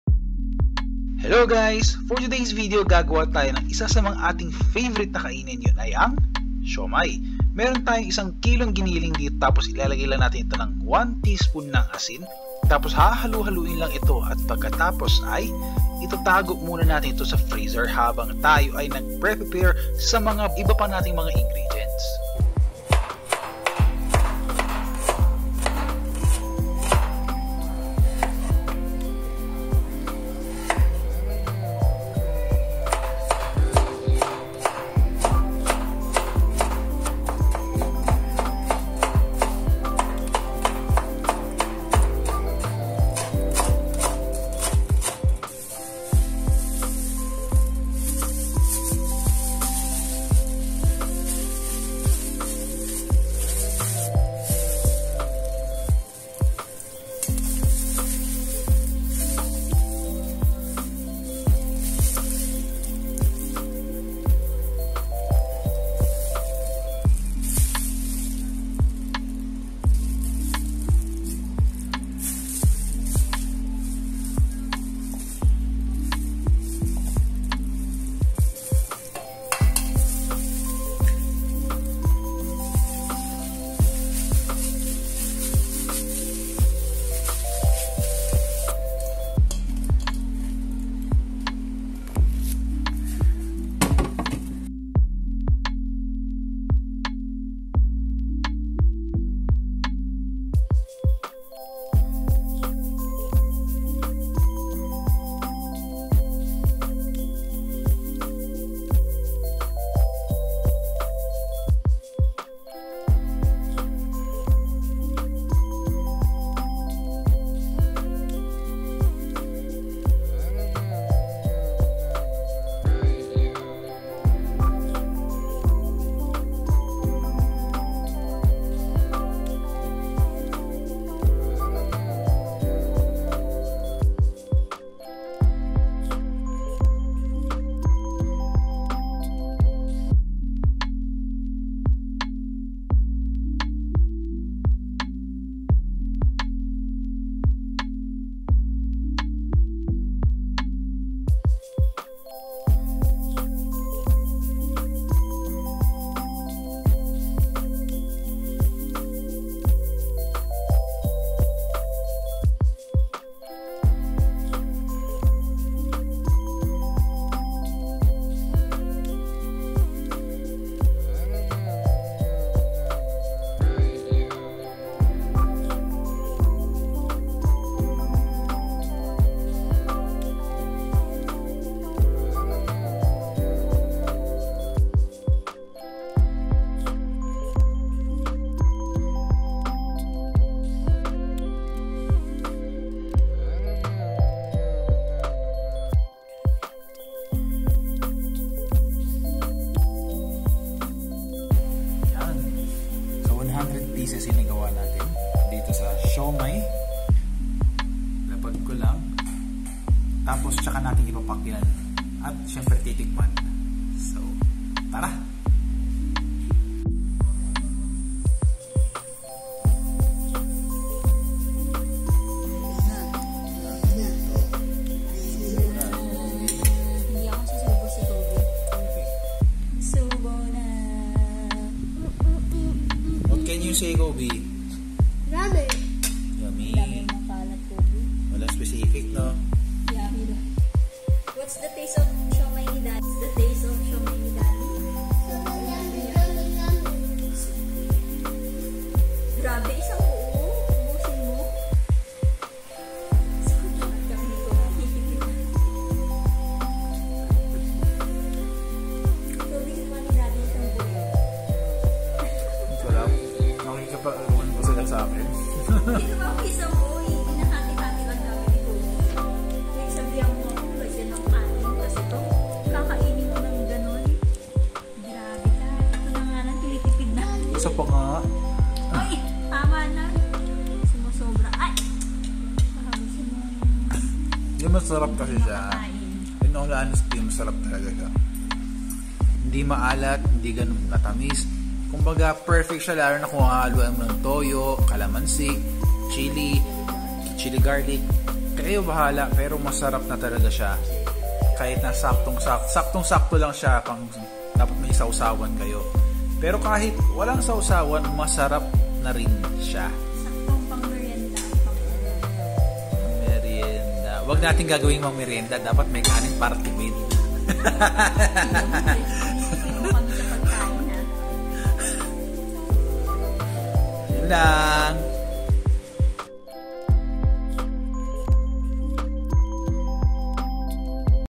Frozen Siomai Recipe pang negosyo o pang baon sa school | Frozen Siomai ni Adel and Kelly
hindi po masarap ang gumawa pero masarap ang nagawa

para po sa mga ayaw na mag gawa pwede po tayo mag order sa amin po
message or comment po

 -Adel and Kelly-